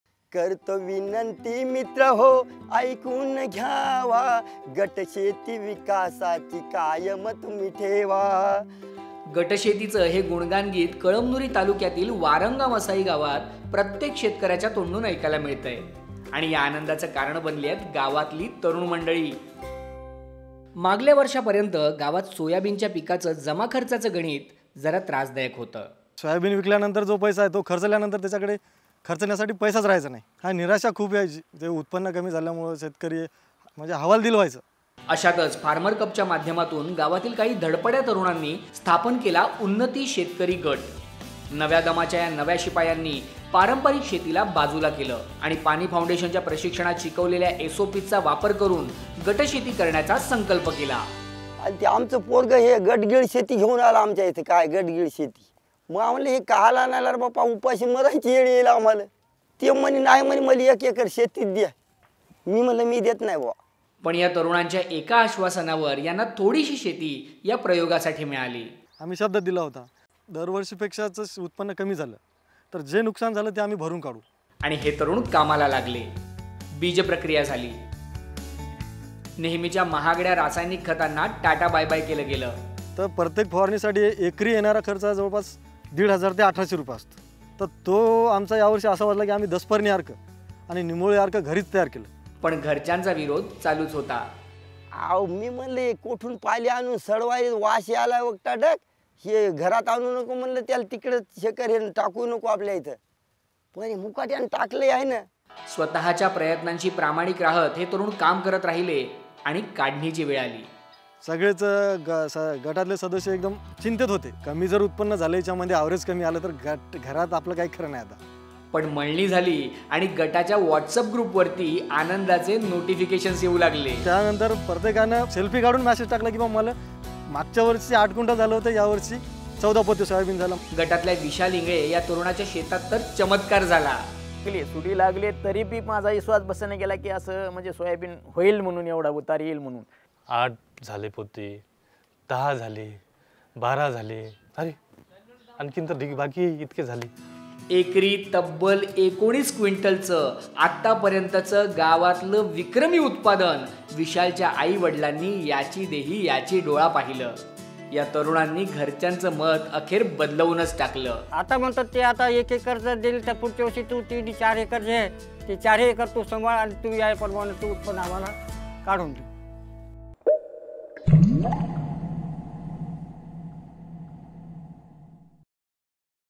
तरुणांनी बदलवलं जेष्ठांच मत! | 19 Quintals Soyabean Per Acre - Young Brigade Brings in Change!
कळमनुरी तालुक्यात वारंगा मसाई या गावात वर्षांनुवर्षे रुळलेली शेती करण्याची पद्धत सोडून गटशेती करण्याला गावच्या जेष्ठांचा विरोध होता. पण गटशेतीतून आलेलं भरघोस उत्पादन पाहून जेष्ठांच मत बदललं. हे कसं घडलं पाहूया या व्हिडिओतून

Varanga Masai village, Kalamnuri taluka: Youngsters in this village proved the older generation wrong when they decided to farm with SOPs and reaped bountiful yields! Watch how.

Join the Paani Foundation community!